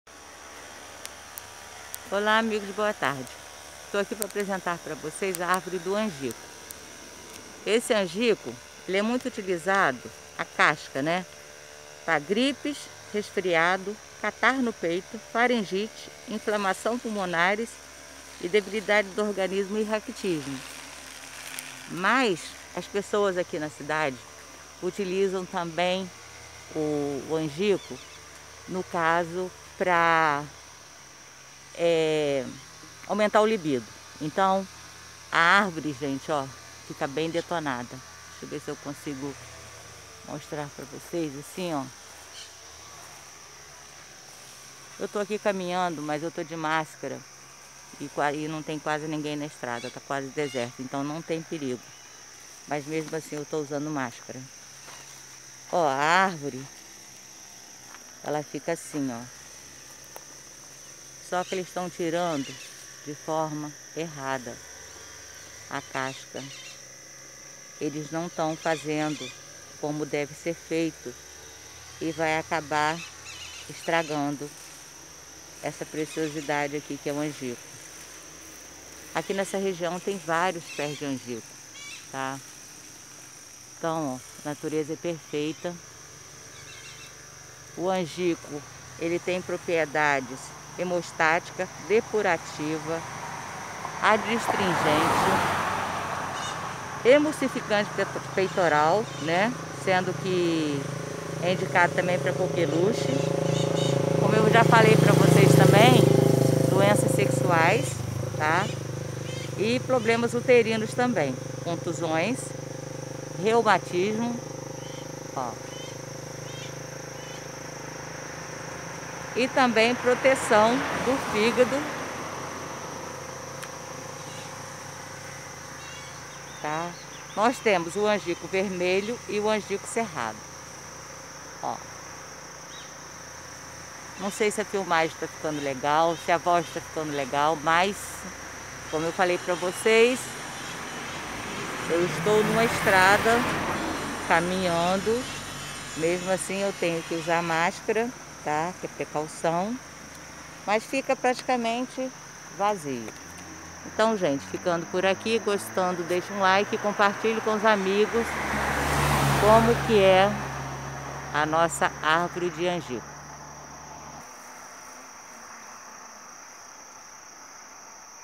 ANGICO E SEUS BENEFICIOS MEDICINAIS !!!!!!
PROPRIEDADES:
HEMOSTATICA
DEPURATIVA
ADSTRINGENTE
CICATRIZANTES
EMULSIFICANTE PEITORAL.

INDICAÇÕES:
TOSSES
COQUELUCHE
DOENÇAS SEXUAIS
PROBLEMAS UTERINOS
CONTUSÕES
REUMATISMO
PROTETOR DO FÍGADO.

COMO PREPARAR O CHÁ:
PARA 1 LITRO DE ÁGUA.
1 COLHER DE SOPA DE CASCA.
COLOQUE A ÁGUA PARA FERVER QUANDO ESTIVER FERVENDO COLOQUE AS CASCAS DEIXE FERVER POR 5 MINUTOS E DEPOIS MAIS 5 EM REPOUSO TAMPADO.
COAR E BEBER 2 XÍCARA DE CHÁ POR DIA.
CONTRA INDICADO PARA:
CRIANÇAS
GRÁVIDAS
E PESSOAS MUITO IDOSA.
OBSERVAÇÃO!:
NO CASO DE DOENÇAS SEXUAIS FAZER BANHO DE ACENTO.
MESMA QUANTIDADE DO CHÁ.